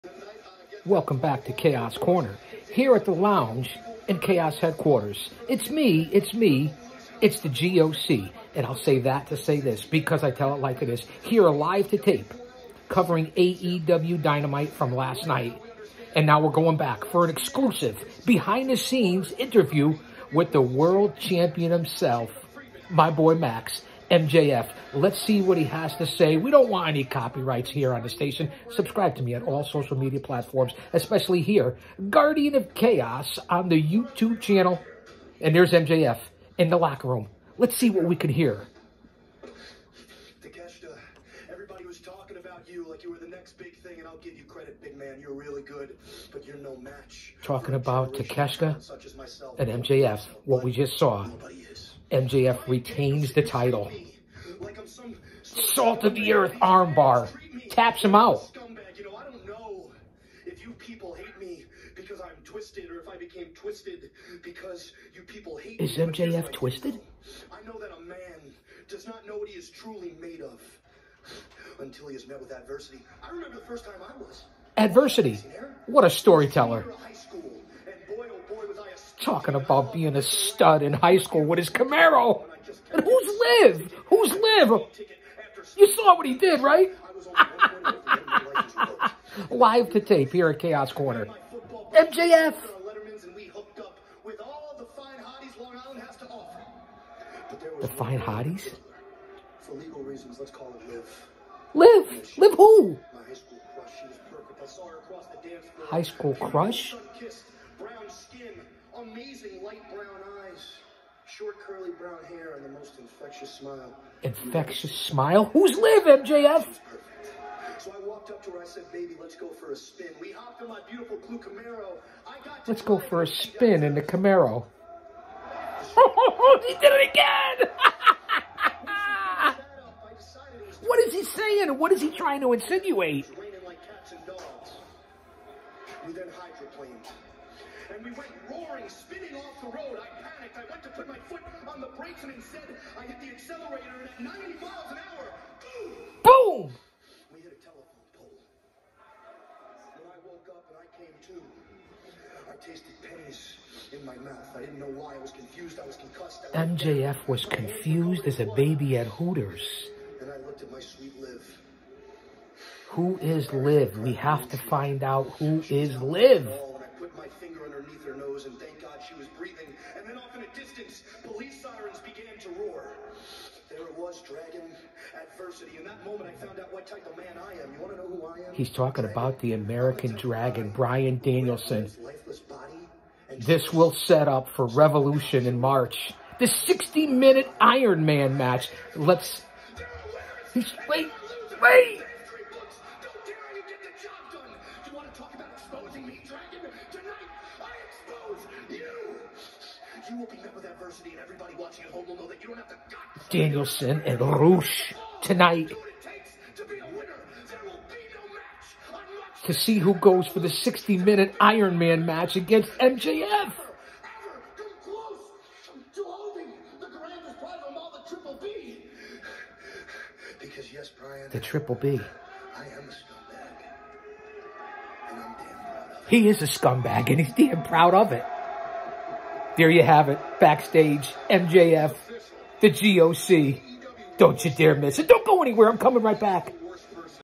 MJF🔥Speaks!(AEW)
prowrestling aewdynamite aew “The G.O.C.🔥Big Daddy” is Live To Tape from the Lounge at “Chaos🎙️Corner” to cover AEW Dynamite